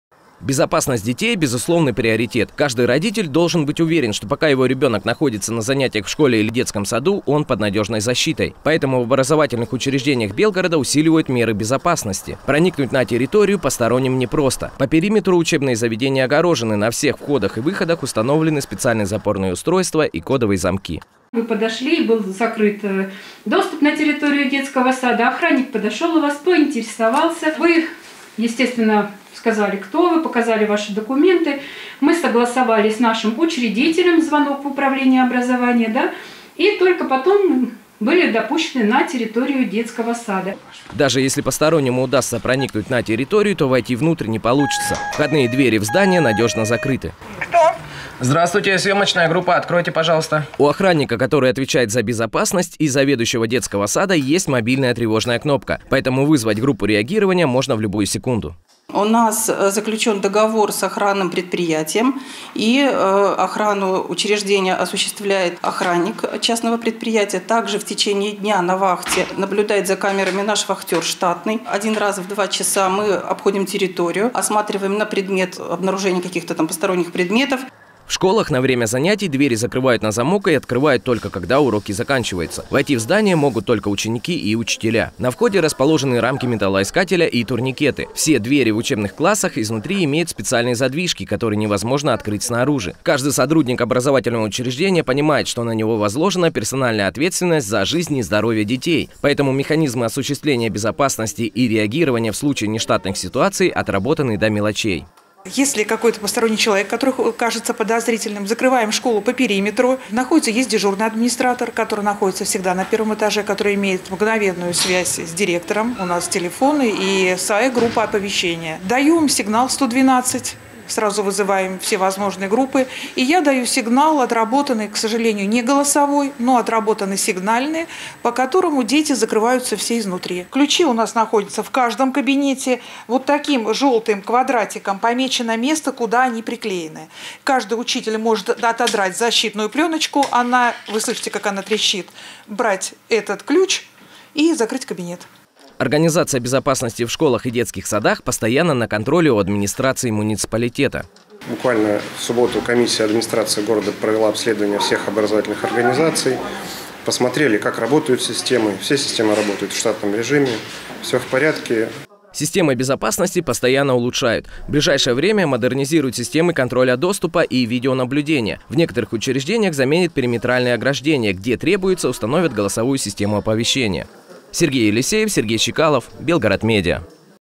В образовательных учреждениях Белгорода усиливают меры безопасности
В ближайшее время модернизируют системы контроля доступа, видеонаблюдения, в некоторых учреждениях заменят периметральные ограждения, где требуется, установят голосовую систему оповещения.